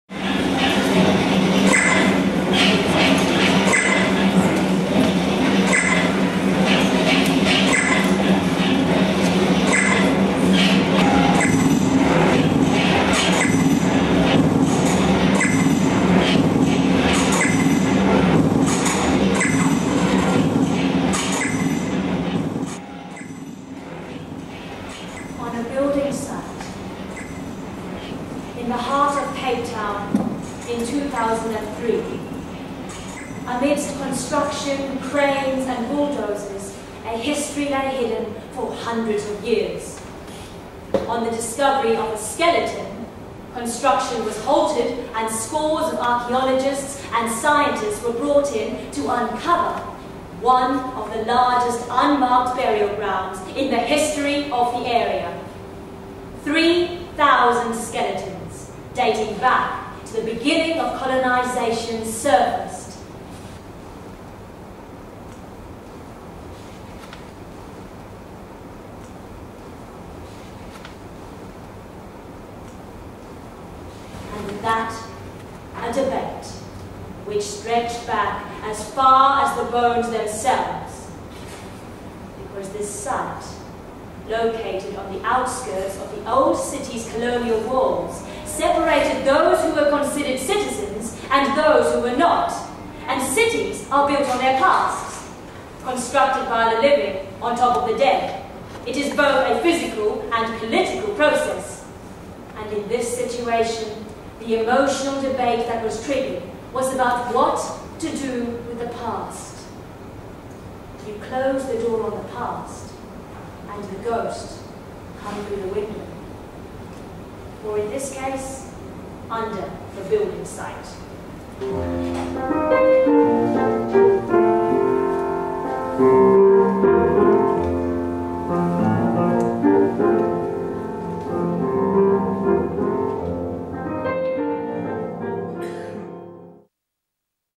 site trailer - Talya Chalef
'site' was inspired by Prestwich Place, a mass burial ground that was discovered on a construction site in Cape Town 2003. Archeologists found that these bodies made up a sum total of approximately three thousand dating back the beginnings of Dutch colonisation. Because this site was located outside the old city's colonial walls, it indicated that they were not of Dutch Reformed origin, setting up an interesting debate.

Using the story from Cape Town set against the backdrop of Australia I was interested in pointing to issues of our shared post colonial condition. We devised and performed the work in the old City Watch House and brought the audience into the space, through an installation and into the ideas.

This was a one-woman performance, which sought to explore all the various perspectives on the situation whilst not dictating or favouring any one specific point of view. The space, sound, projections, movement and text all contributed equally to the piece.